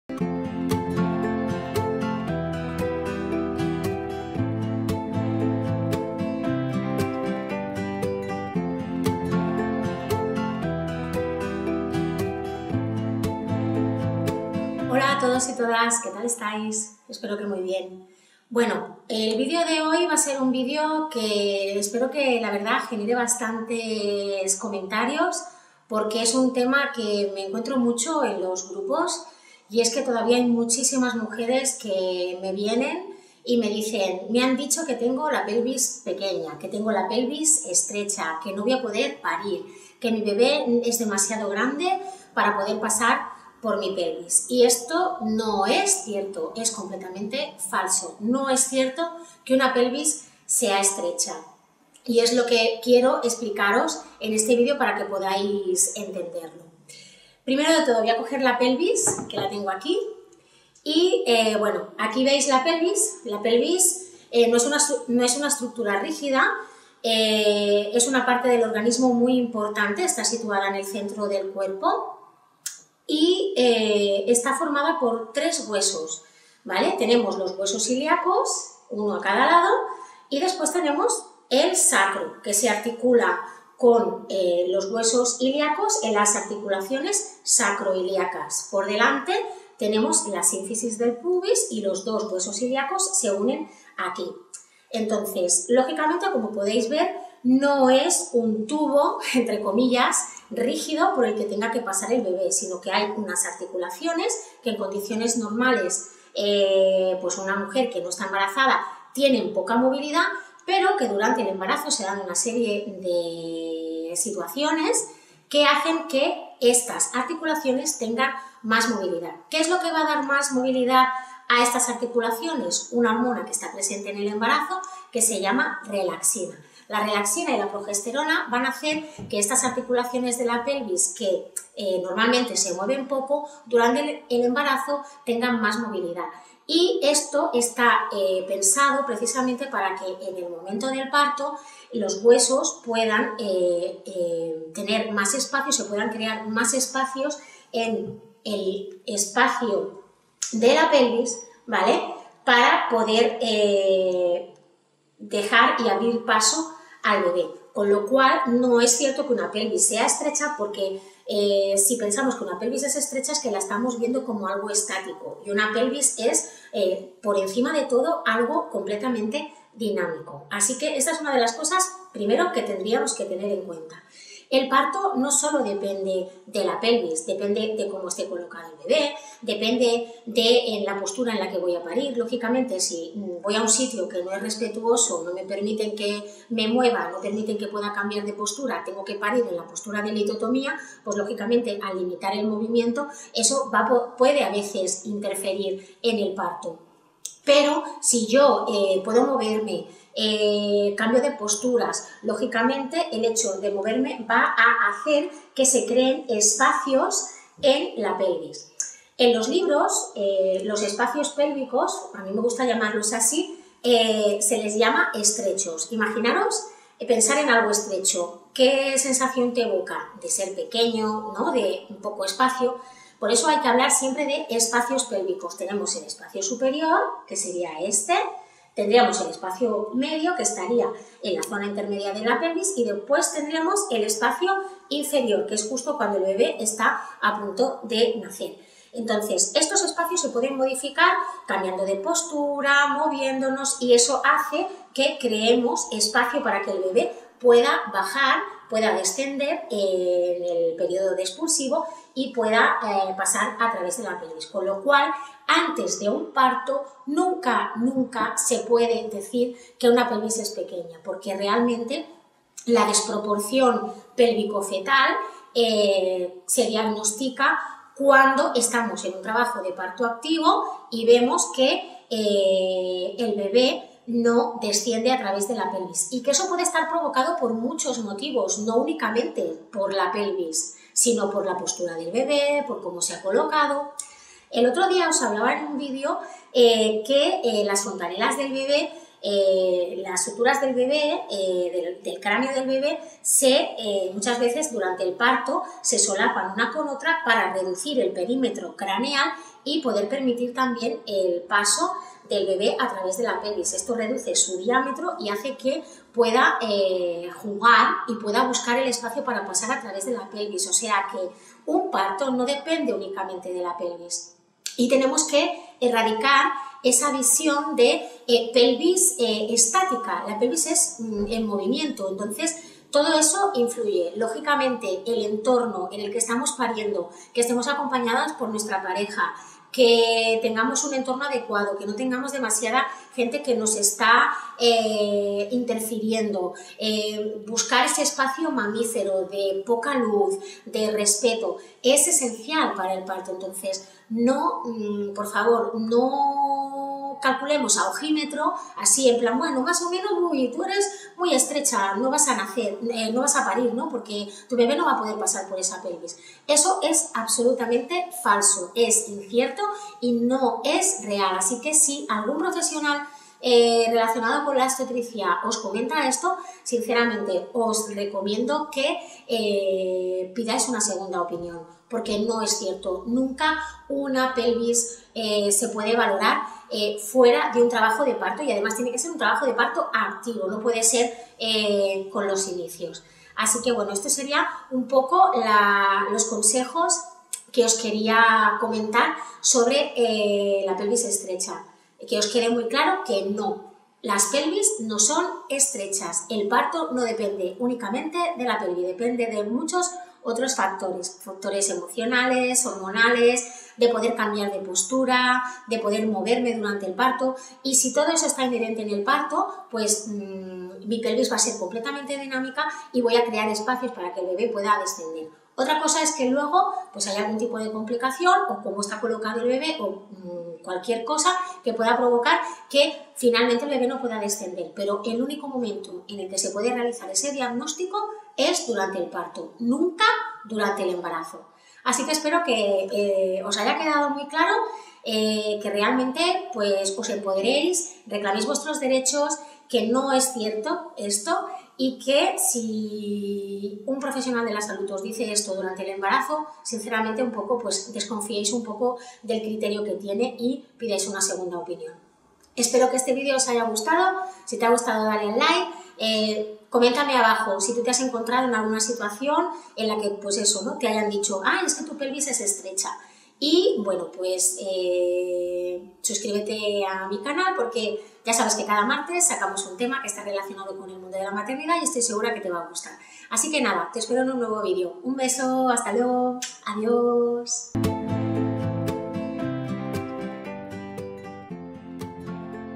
¿PELVIS ESTRECHA? · ¿Podré PARIR? ¿Me harán una CESÁREA?
Aún hoy en día algunos profesionales indican que algunas mujeres no podrán parir vía vaginal porque tienen la pelvis estrecha, pero esto no es cierto, ya que solo se puede diagnosticar una  desproporción pélvico-fetal durante el trabajo de parto activo.

El parto depende de muchos factores no únicamente de la pelvis, además la pelvis es dinámica tiene articulaciones que le permiten moverse durante el parto para crear espacio para el bebé, del mismo modo que la cabecita del bebé se adapta al diámetro de la pelvis gracias a las fontanelas.

Por todo esto, el movimiento durante el parto es esencial, es una forma de ayudar a tu bebé a nacer.

¿Sabías que existe una hormona durante el parto que permite una mayor elasticidad y movilidad de la pelvis? El cuerpo es sabio y lo tiene todo pensado. Os recomendamos mucho movimiento que os ayudará de cara al parto, en nuestras clases os enseñamos qué ejercicios pueden ayudarte a abrir el espacio pélvico. ¿Te animas?

Si quieres saber más, no te pierdas este vídeo.

▷¡SÍGUENOS!

▷ Melodía de inicio creada por Silvia Molina con programa Music Maker JAM